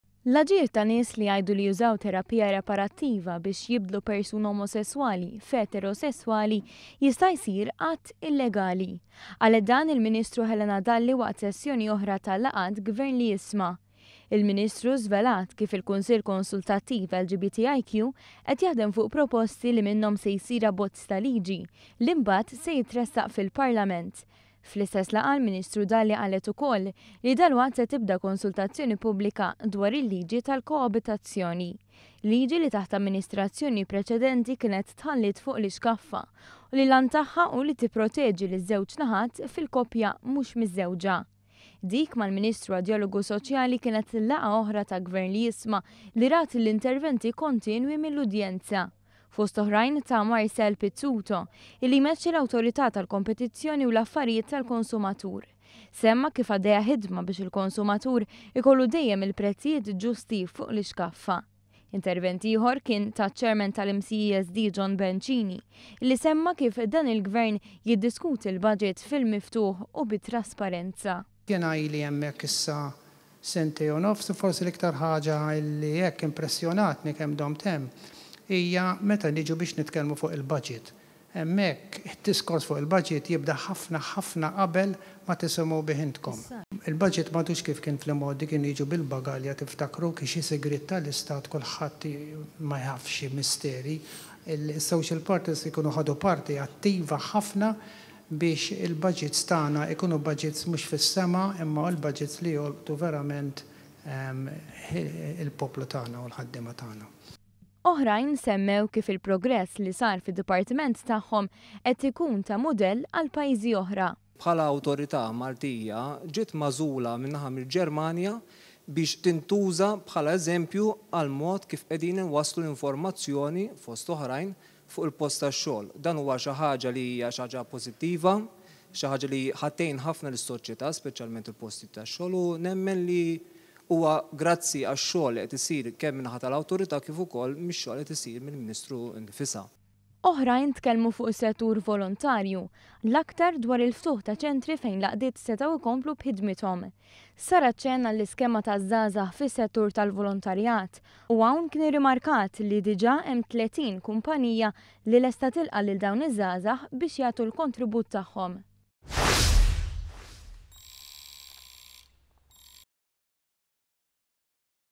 Jispikka d-djalogu soċjali u d-drittijiet.
Waqt laqgħa oħra mis-sensiela Gvern li Jisma', l-Ministru Helena Dalli ħabret żewġ affarijeit li se jaffetwaw lis-socjeta’ partikolarment persuni LGBTI u koppji li qed jgħixu flimkien iżda mhemmx xi tip t’irbit.